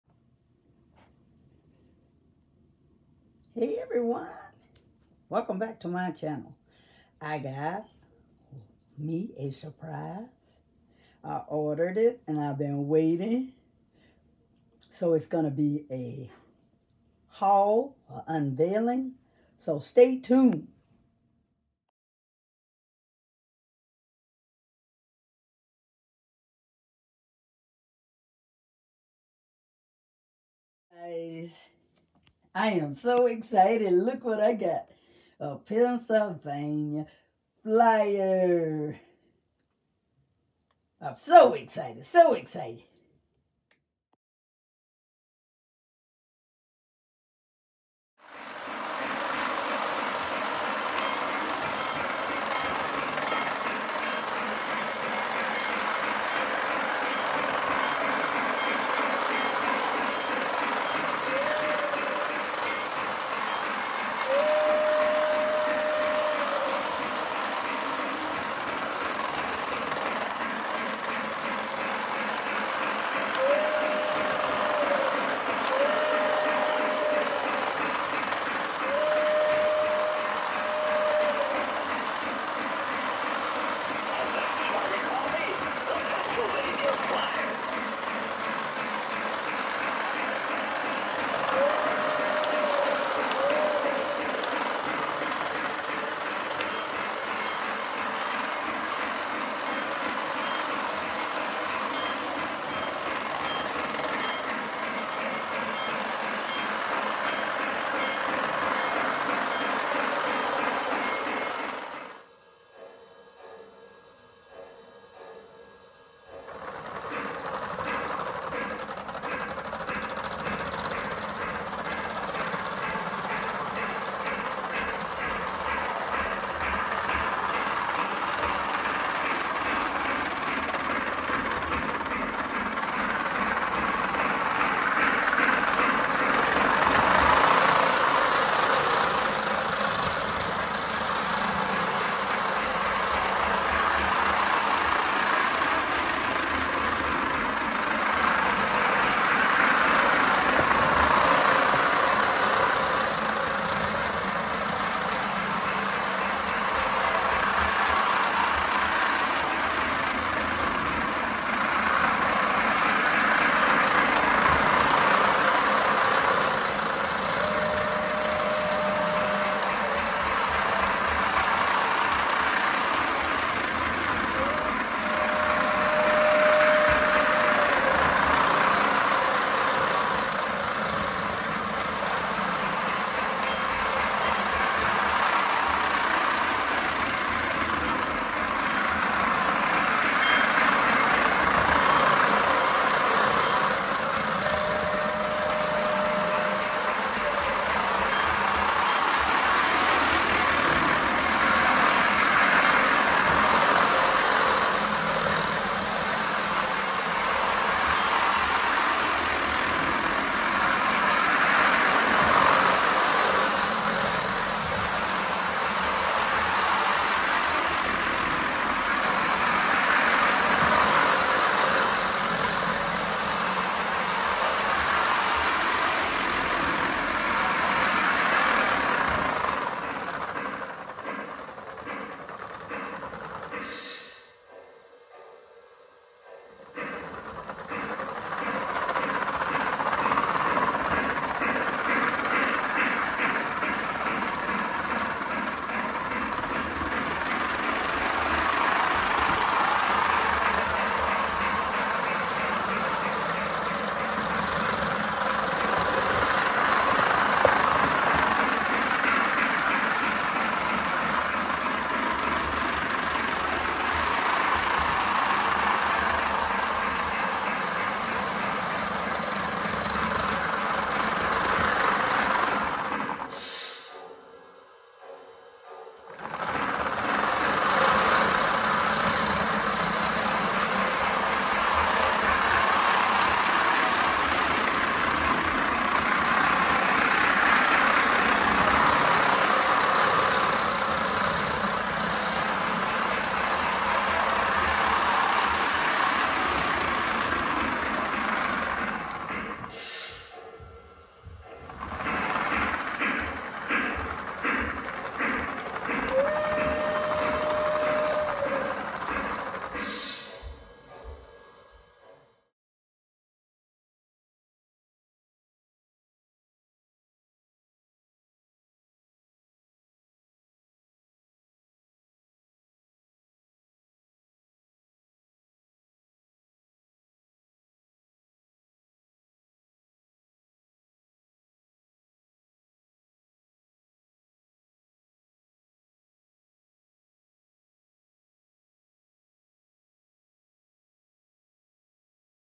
My First Model Train it a Lionel Pennsylvania Lion Chief Set
My First Model Train it a Lionel Pennsylvania Lion Chief Set with Blue tooth
capability . With electric 0 tracks, unscented smoke fluid, Remote control and you
can use your phone with the Lionel app, locomotive and steam box, Tank Car , Bow-
Car ,and a Caboose  + tracks with Voice streaming and recording. The train travel forward
and Backward low to high speeds.you can purchase a Lionel train at Amazon, HSN ,QVC
and at Lionelstore.com and other various stores. I am so Excited about the train.